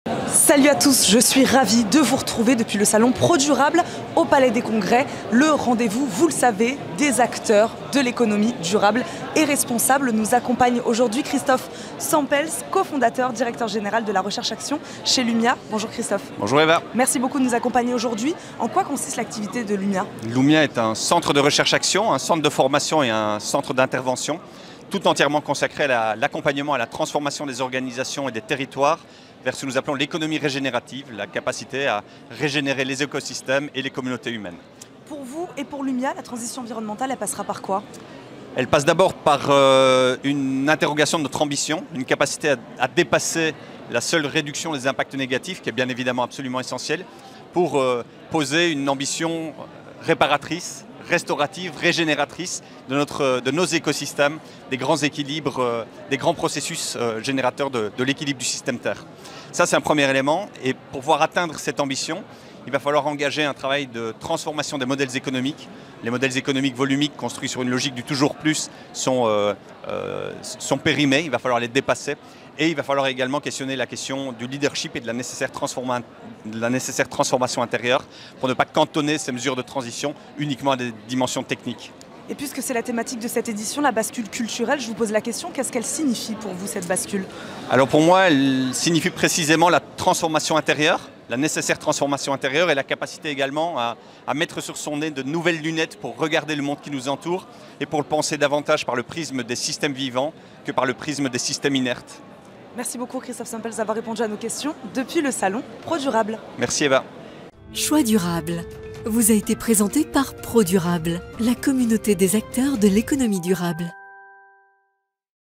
CHOIX DURABLE - Interview : Christophe Sempels (LUMIA)
Mercredi 13 septembre 2023, CHOIX DURABLE reçoit Christophe Sempels (Directeur Général de la recherche-action, LUMIA)